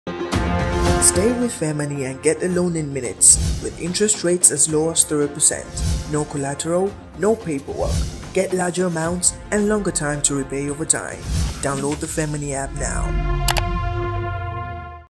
Stay with FairMoney and enjoy as low as 3% interest rate on your loan!
You can enjoy as low as 3% interest rate on your loan and longer repayment plans when you stay with FairMoney.

FairMoney is a digital platform  that allows you access instant loans and perform bank transactions in a few minutes through the FairMoney app.